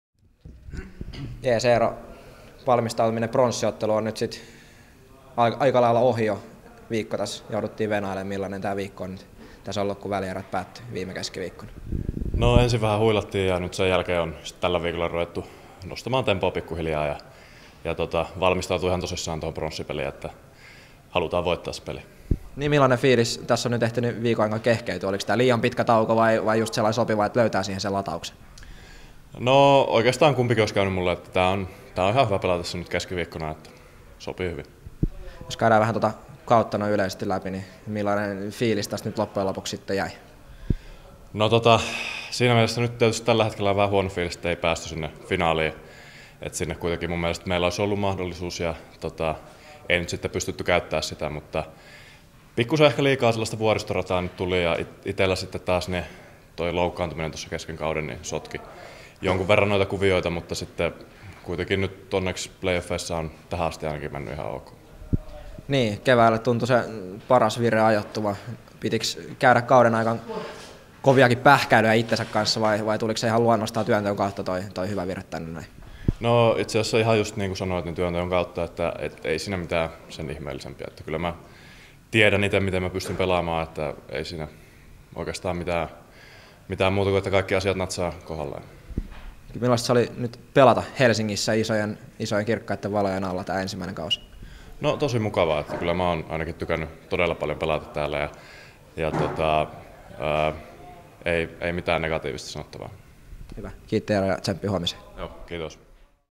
Kilpeläinen ennen pronssipeliä
Eero Kilpeläinen haastattelussa ennen pronssiottelua Bluesia vastaan.